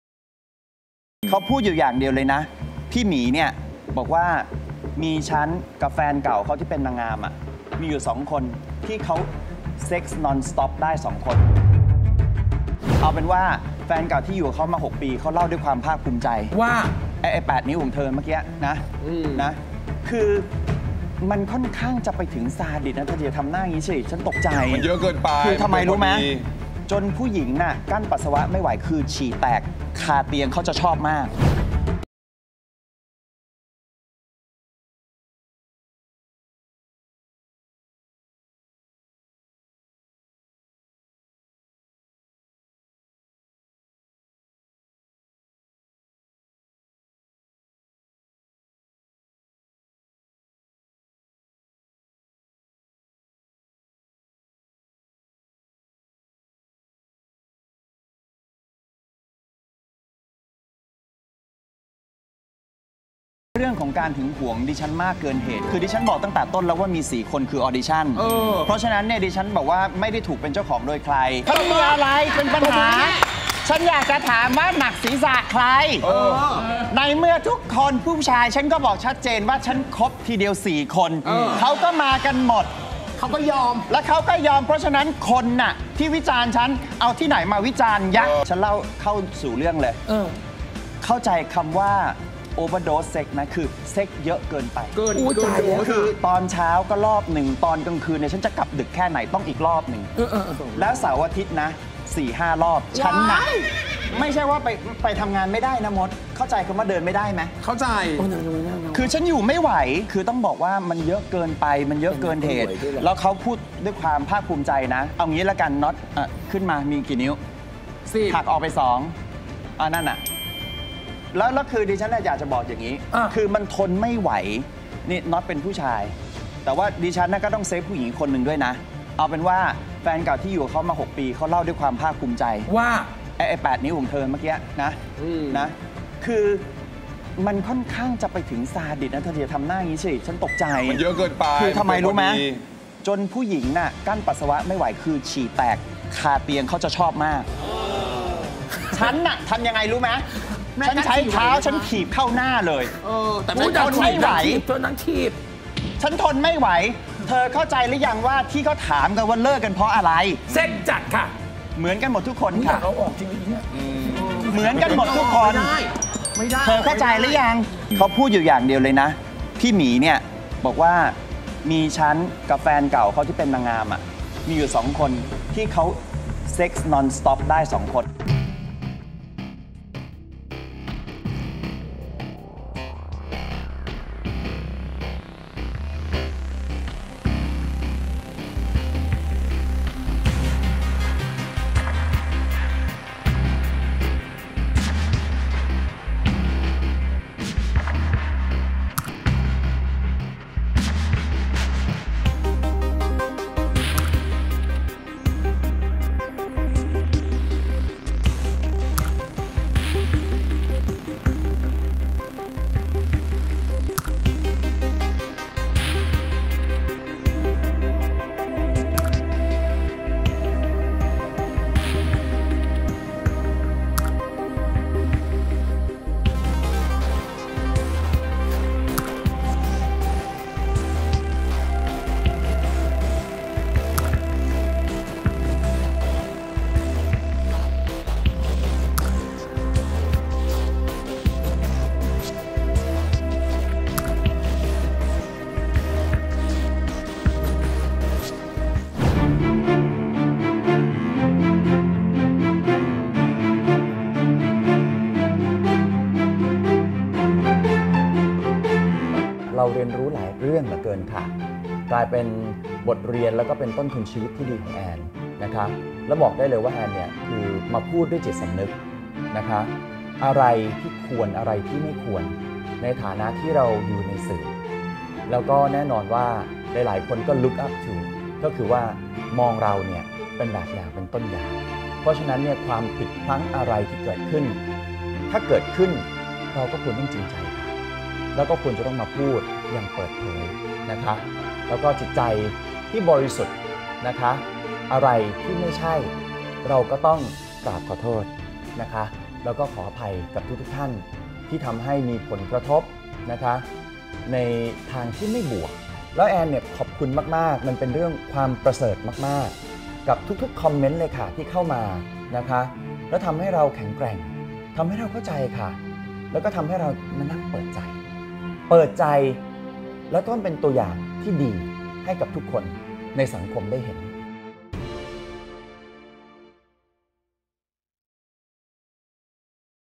แอน จักรพงษ์ ทัวร์ลงหนัก ชาวเน็ตแห่แบนหลังสัมภาษณ์พาดพิง แคทรีโอนา เกรย์ Miss Universe 2018 | SPRiNG
แอน จักรพงษ์ งานเข้า! โลกโซเชียลติดแฮชแท็กแบน หลังสัมภาษณ์ไม่ให้เกียรติ แคทรีโอนา เกรย์ Miss Universe 2018 ผ่านรายการแฉ หลังเลิกรากับแฟนหนุ่ม คลินท์ บอนแดด หรือ พี่หมี อดีตแฟนเก่านางงามจักรวาลคนดัง เพราะทนความเซ็กส์จัดซาดิสม์ และนางงามจักรวาล ก็เลิกราเพราะเหตุผลนี้